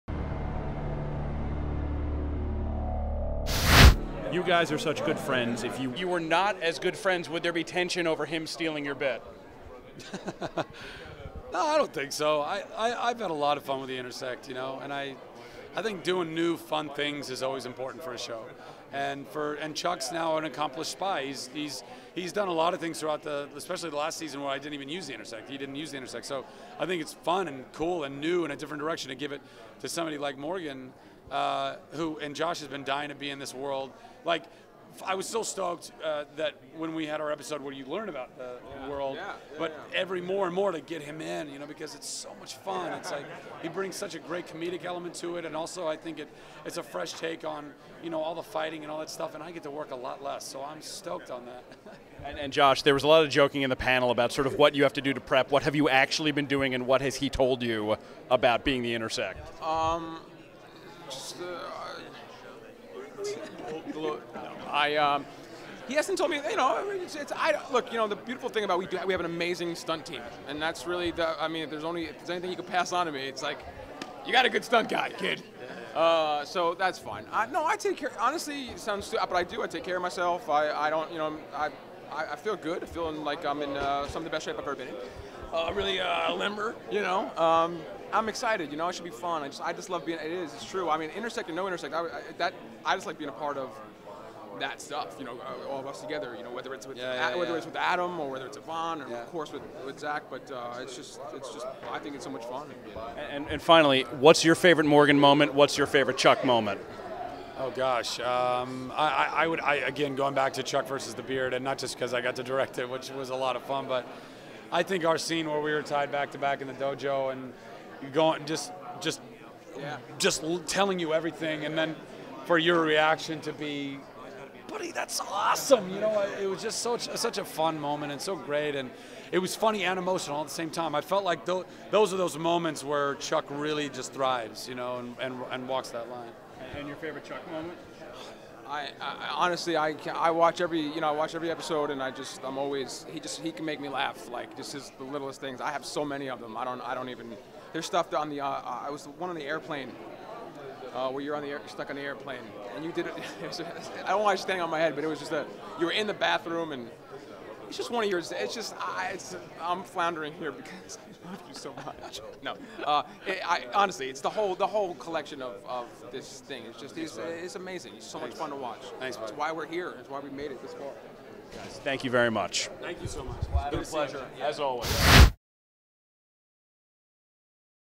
Chuck - Joshua Gomez and Zachary Levi
Joshua Gomez and Zachary Levi talk to Alan Sepinwall about the final season of Chuck